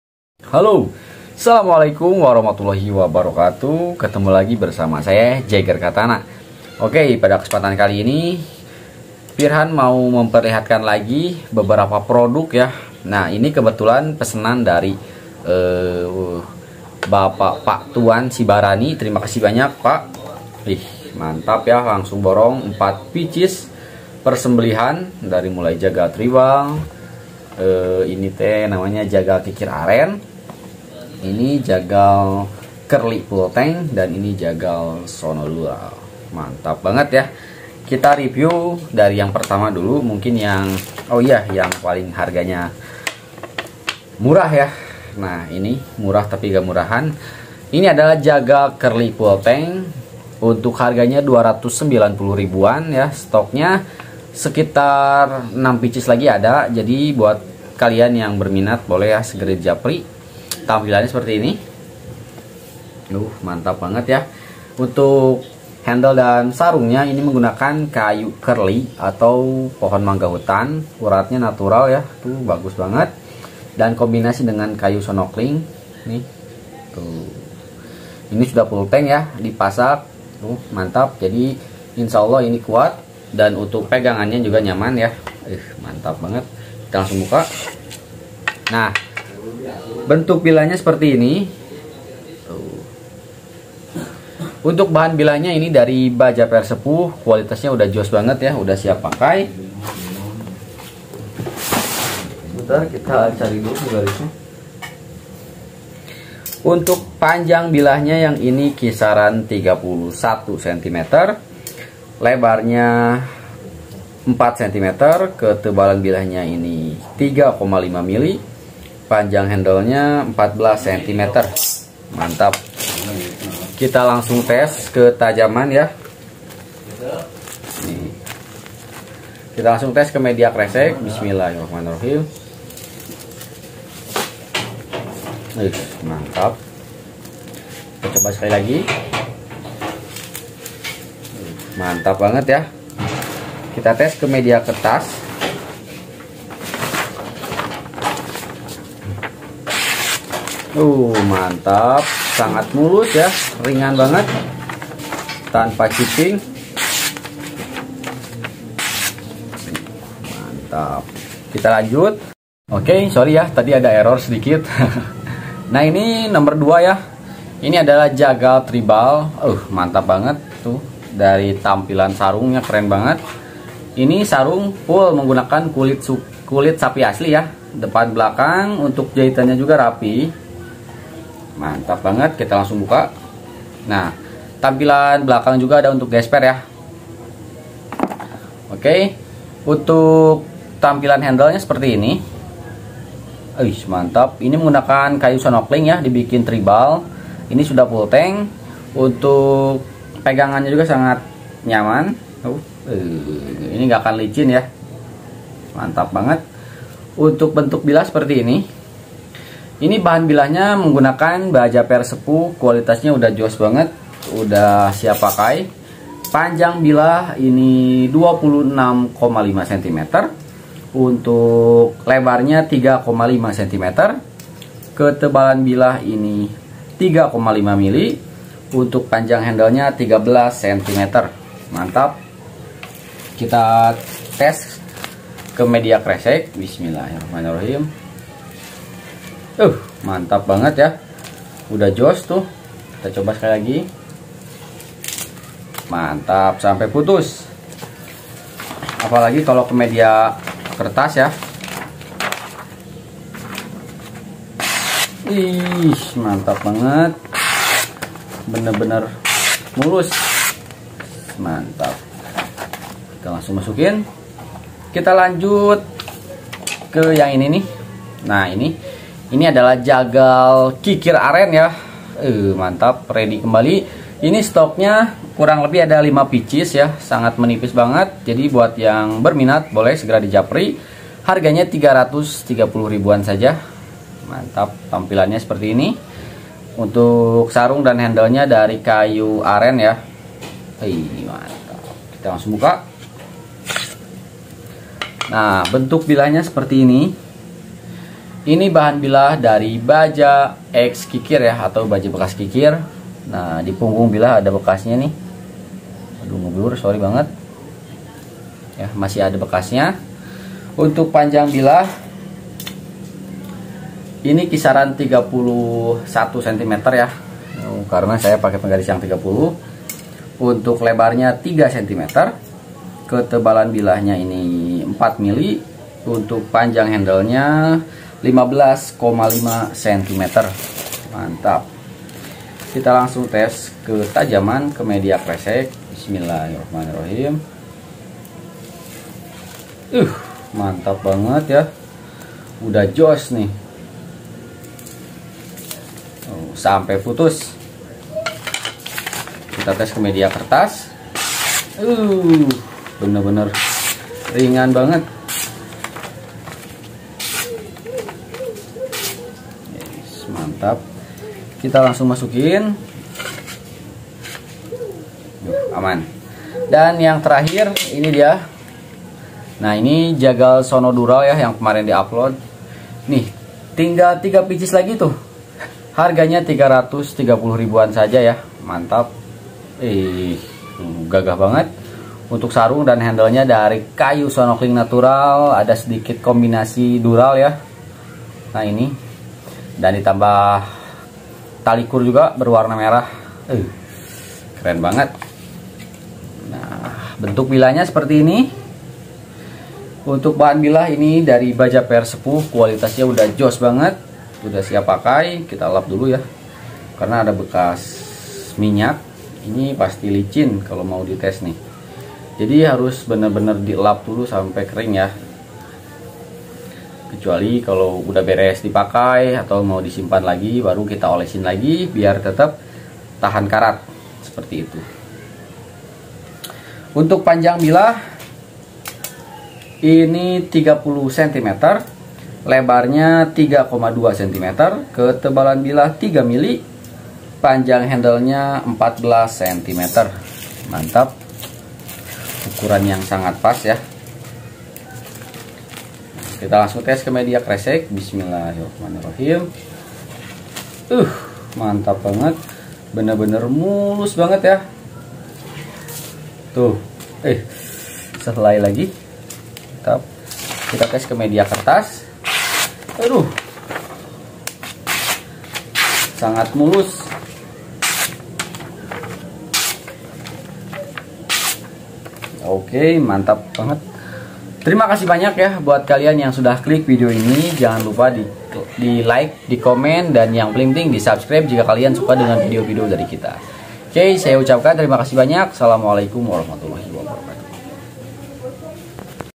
BORONGAN 4 PCS MELUNCUR ke RIAU // PAJAGAL SEMBELIH
1. Laduk Sembelih ukir : 290k
2. Jagal tribal : 330k
3. Jagal kikir aren : 330k
4. Jagal Sono dural : 330k

Untuk yg berminat langsung aja di japri ke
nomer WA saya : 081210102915